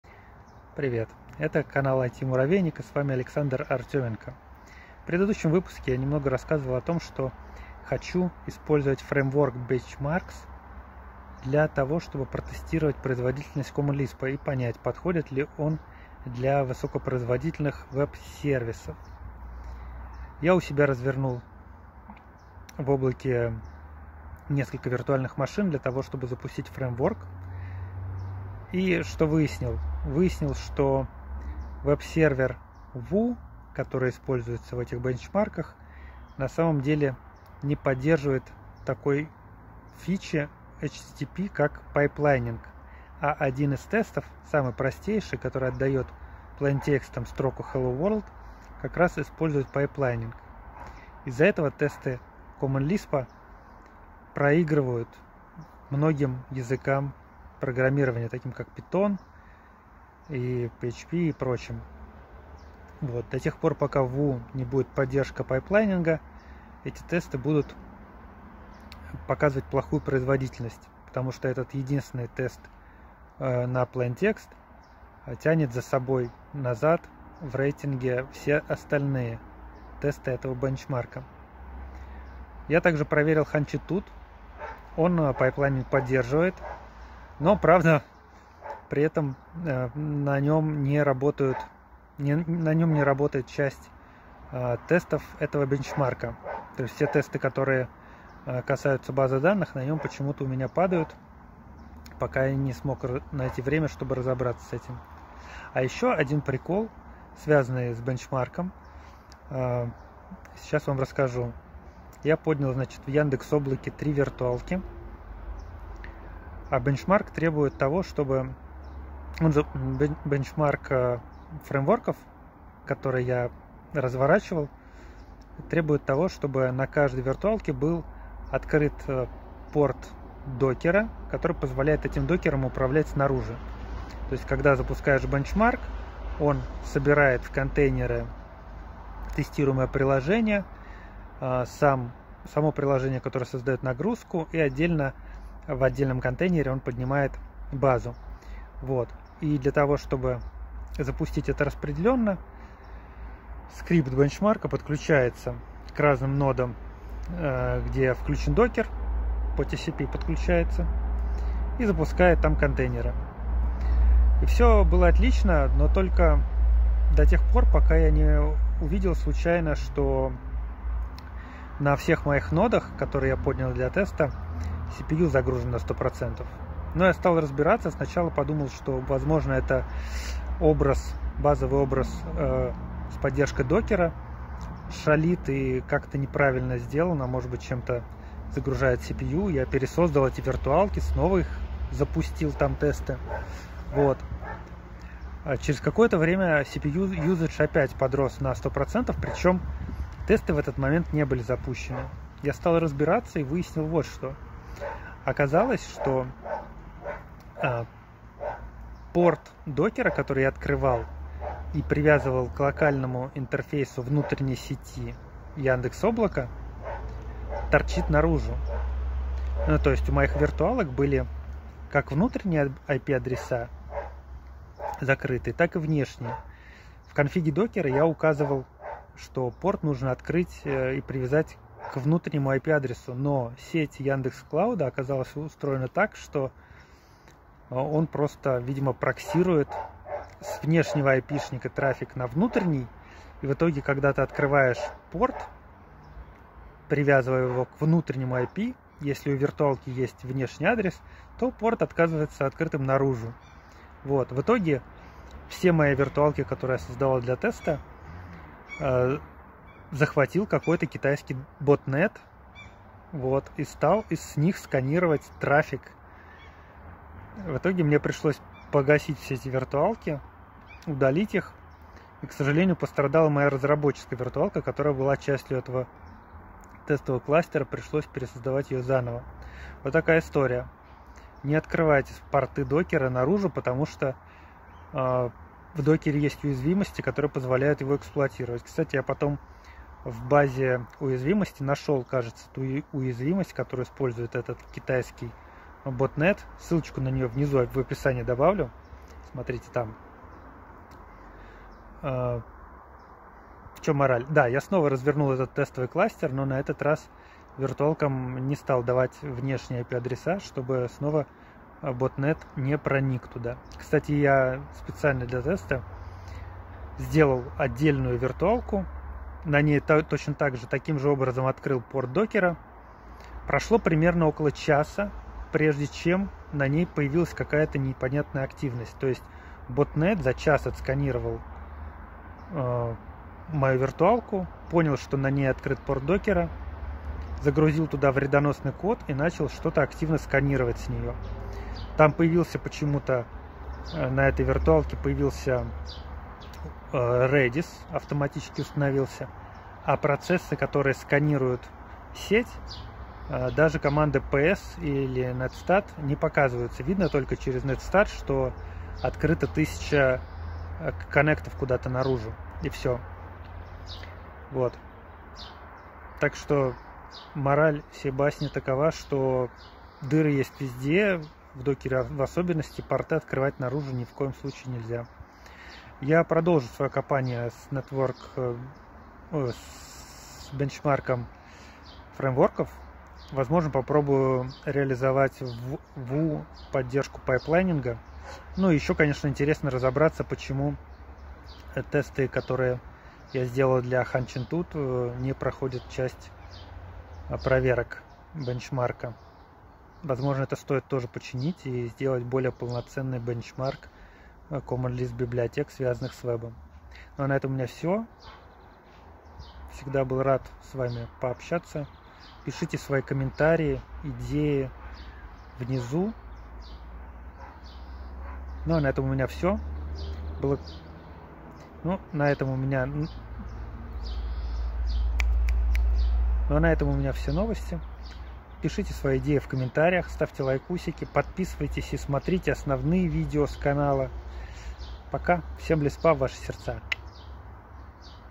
VLOG#2 How I've tested a Common Lisp and become a part of Chineese botnet!
Рассказываю, как я тестировал производительность Common Lisp вебсервера Woo на  и стал жертвой китайского ботнета.

*Ссылки*:

- а вот этот эксплоит он скорее всего использовал для выхода из песочницы Docker